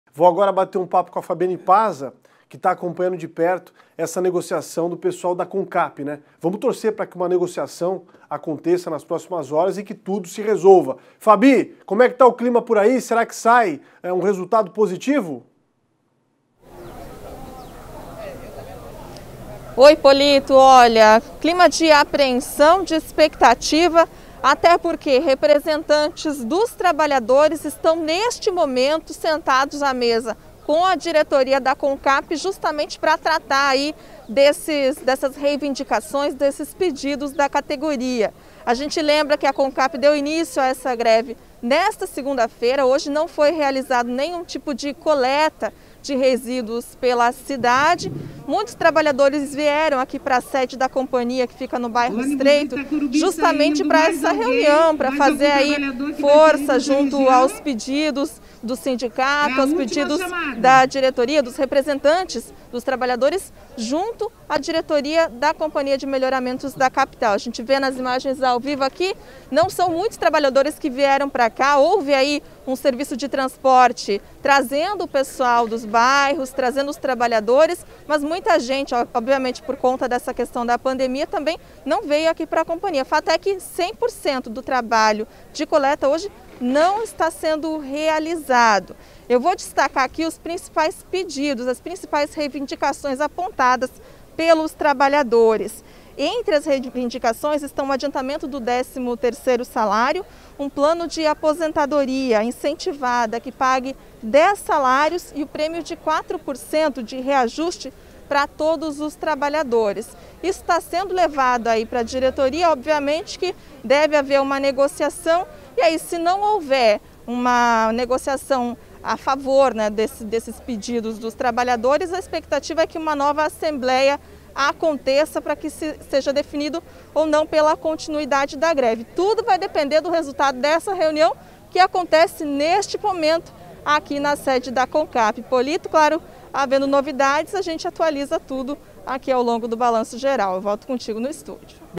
Comcap: greve suspende coleta de resíduos nesta segunda-feira
A coleta de lixo foi suspensa na manhã desta segunda-feira (15) por causa da greve dos trabalhadores da Comcap.

A equipe do Balanço Geral se deslocou até o local para ver como estão as negociações. Confira as informações completas.